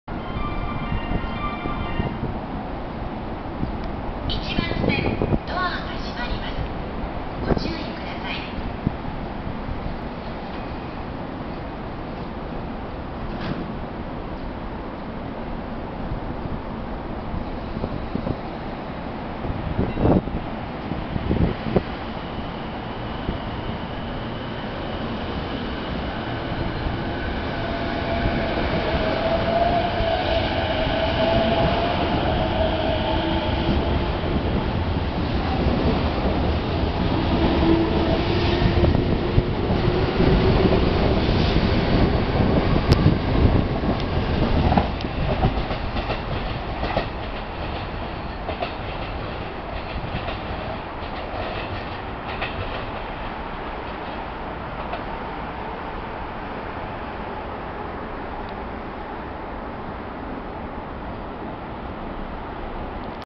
JR京葉線 葛西臨海公園駅1番ホームからE233系5000番台各駅停車が発車
2018年4月20日撮影

JR京葉線　葛西臨海公園駅1番ホームからE233系5000番台各駅停車が発車します。